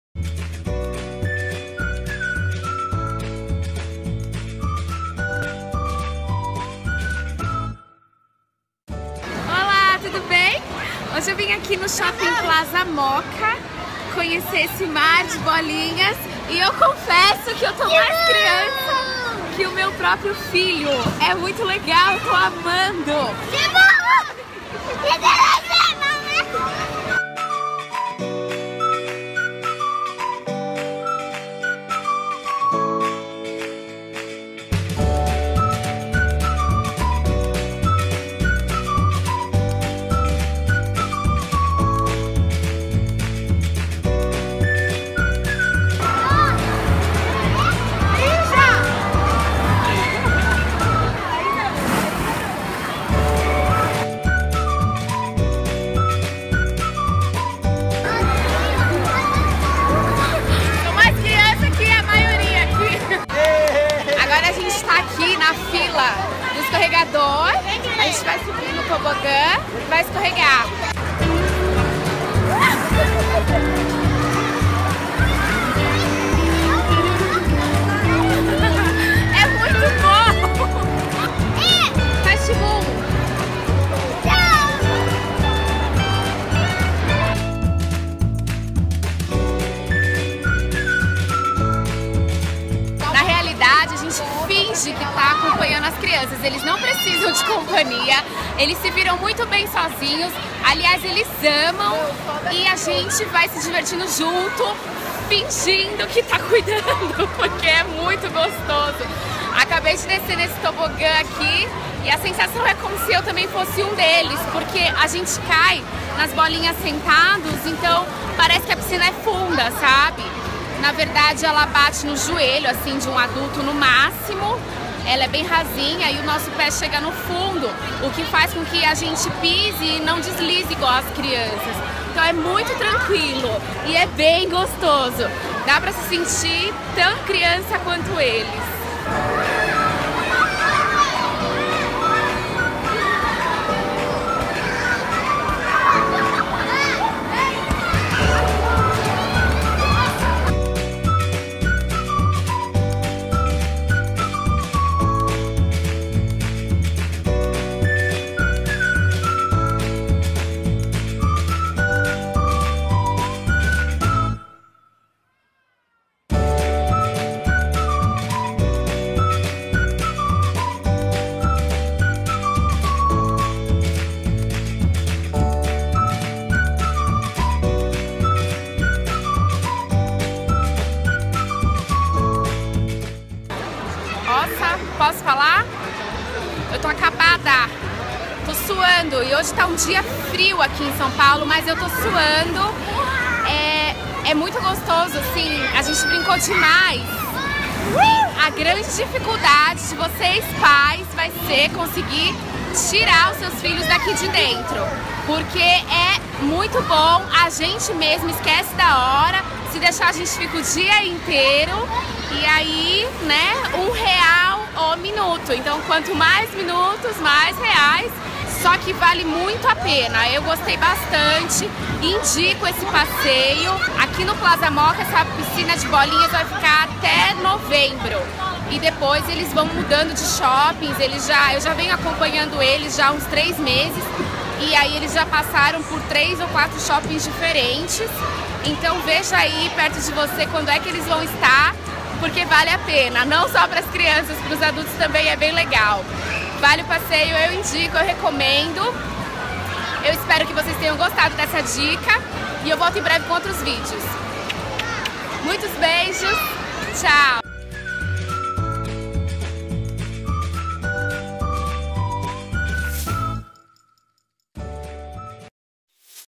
Mar de Bolinhas - Piscina de bolinhas gigante
Estive com meu filho na piscina de bolinhas gigante que tem andado pelos shoppings de São Paulo e NÓS (ele e eu) amamos!!!

Um passeio que vale cada centavo e pode ser curtido em família.

Um ótimo programa de dia das crianças!!!